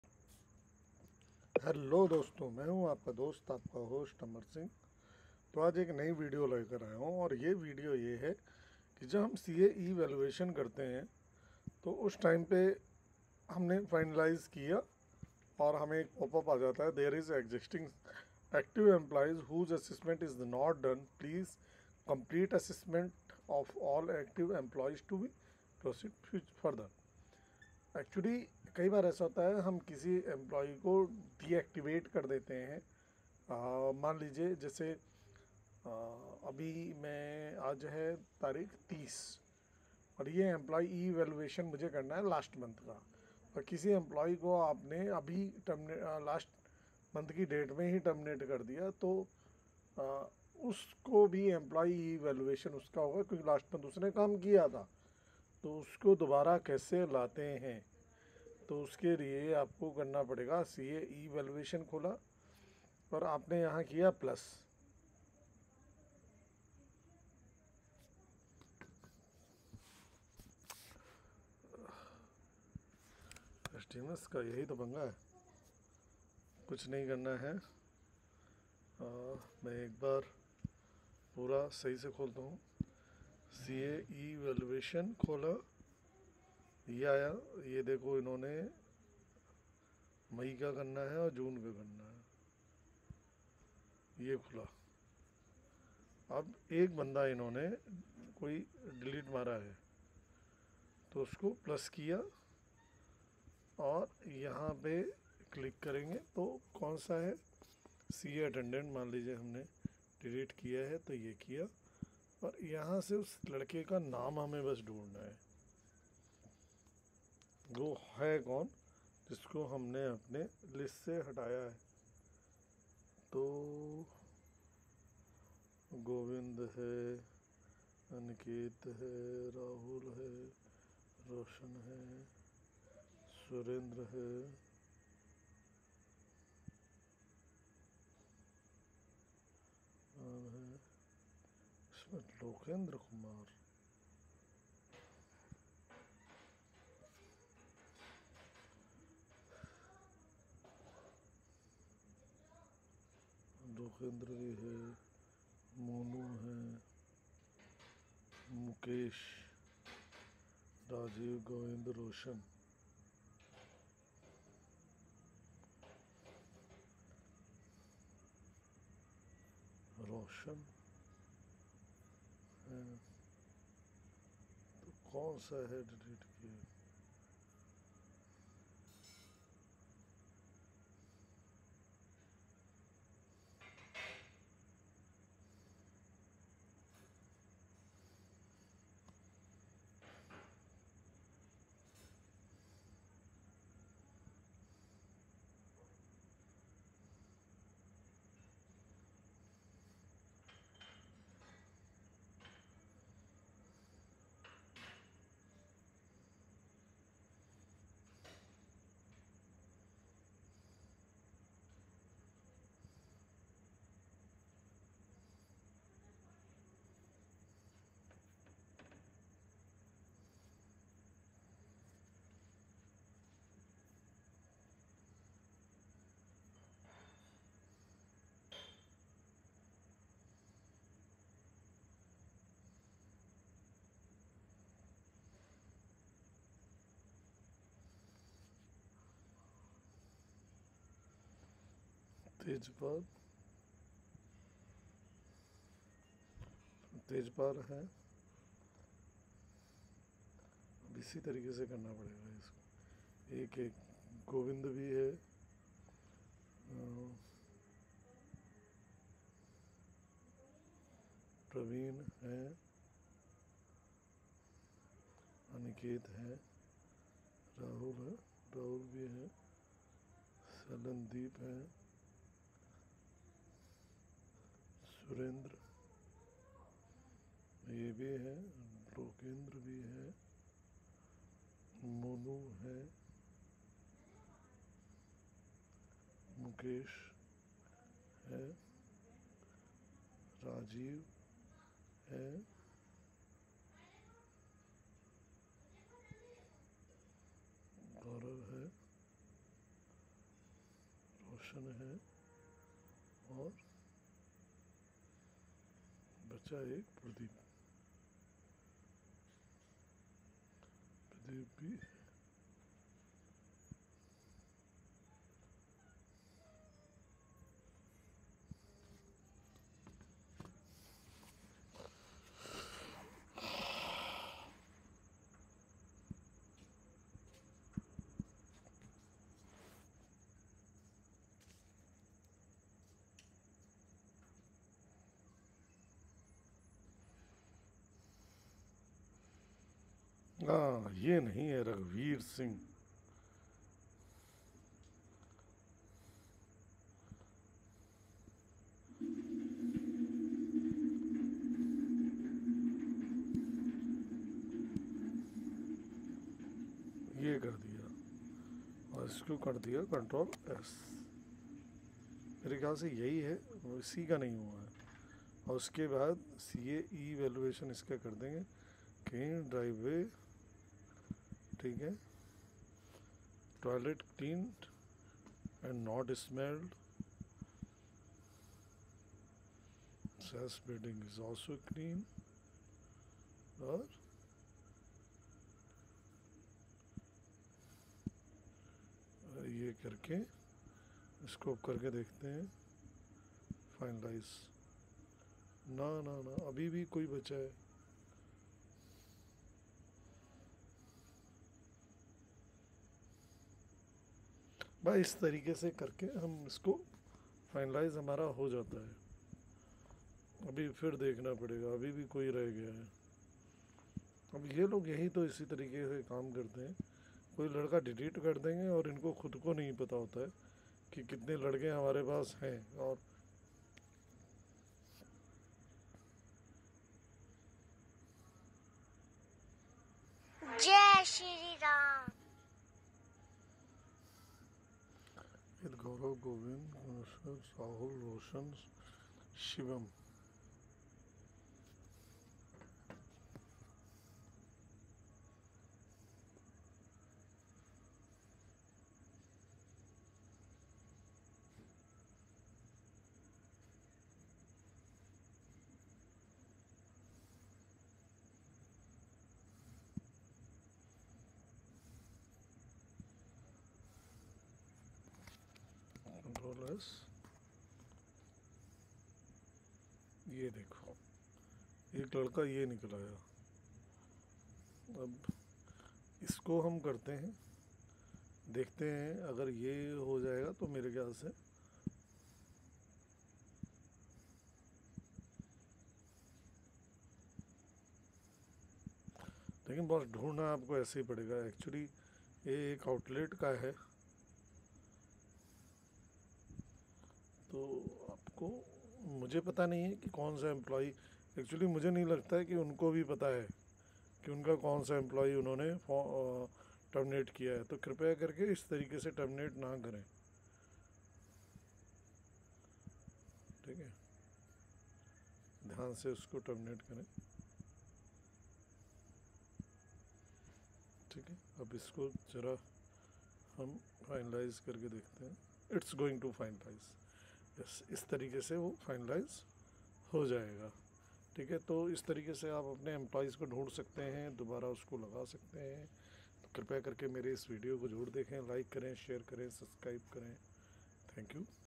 जब employee evaluation करने से पहले कोई CA को Terminate कर देते है तब Evaluation करना
@indianoilsdms @Infobite_india @employee evaluation @Sdmsindaneportal @indianoilcorporationlimite2332 @Indianoil427 @indianoilsdmssupport6167